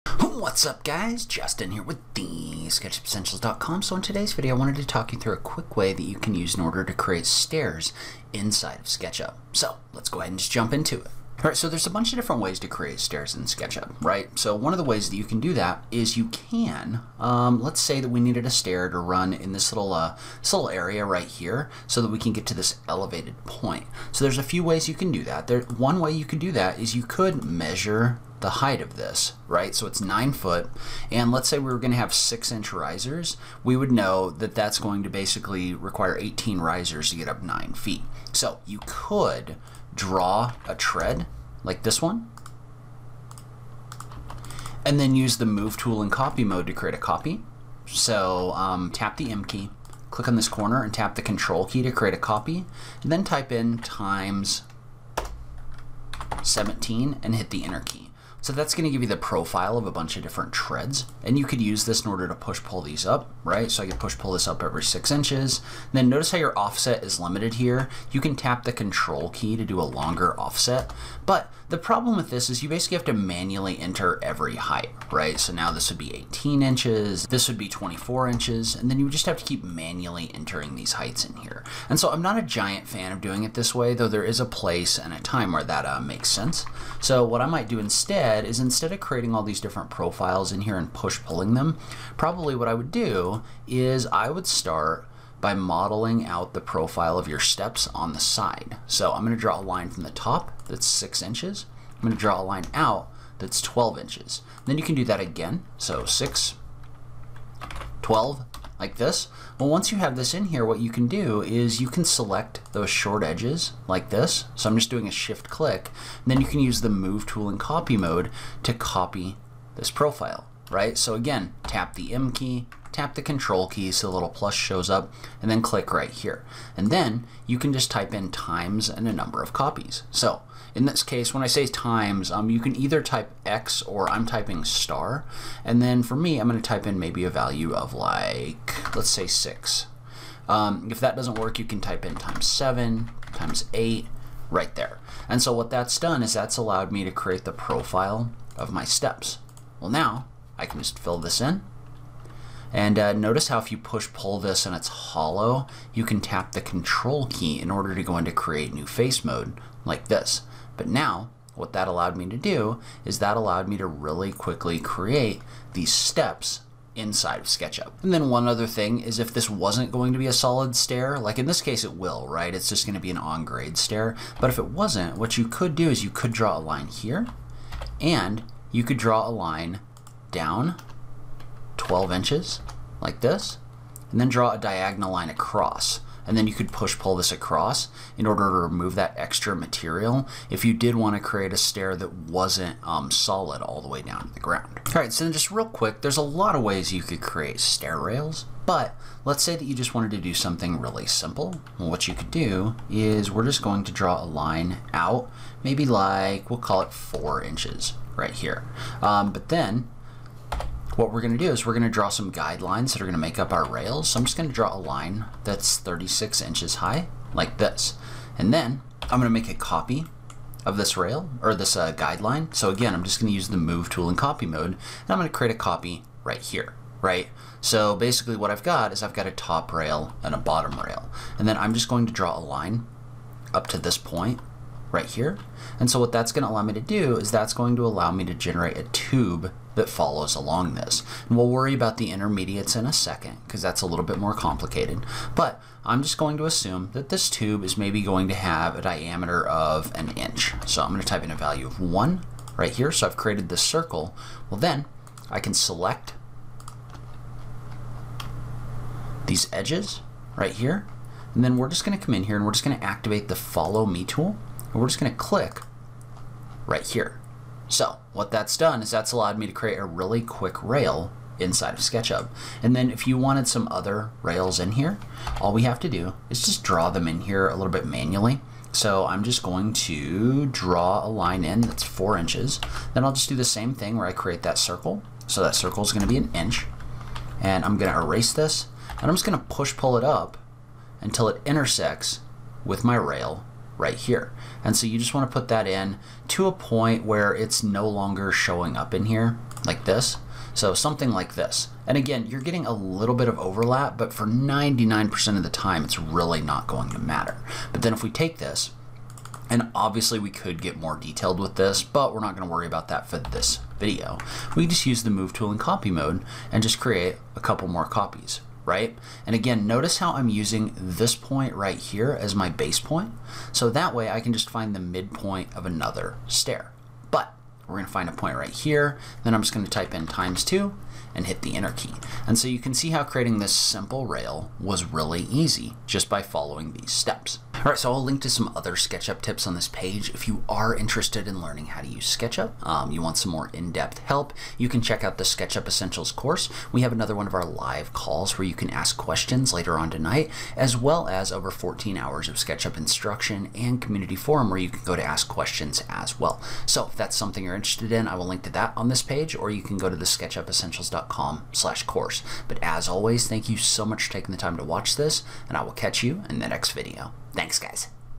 The FASTEST Way to Create Stairs in SketchUp! (Plus - Making a Rail!)
In this video, learn the fastest way to quickly create stairs in SketchUp!

TIMESTAMPS
0:00 - Introduction
0:15 - Setting up your stairs to model in SketchUp
1:35 - The Fastest way to create stairs
2:55 - Making the base of the stair sloped instead of solid
3:15 - A fast way to add pipe rails to your stair
5:38 - Learn SketchUp in the course!